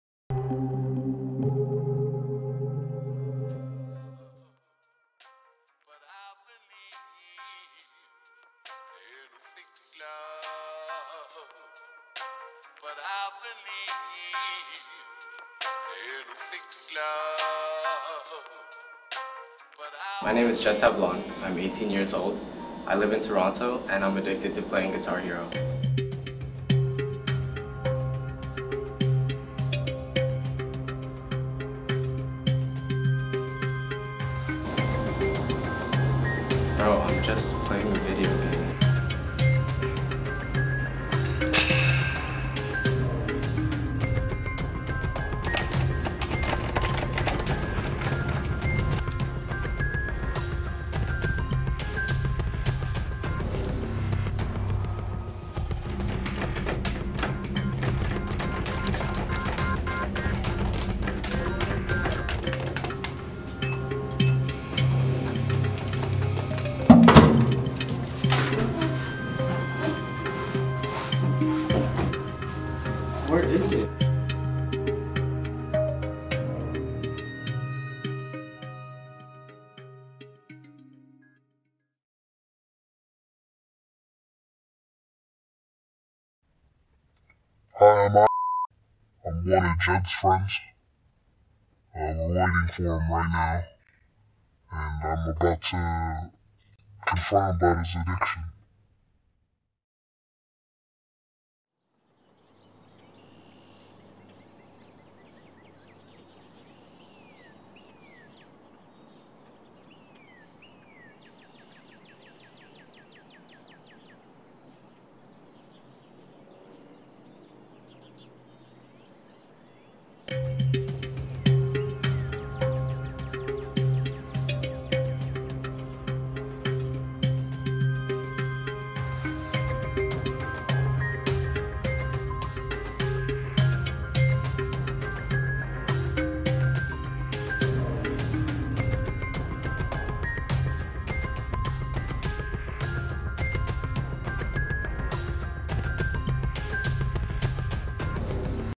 My Strange Addiction (parody): Guitar He​ro
Jed has been playing for 4 years, 20 hours everyday.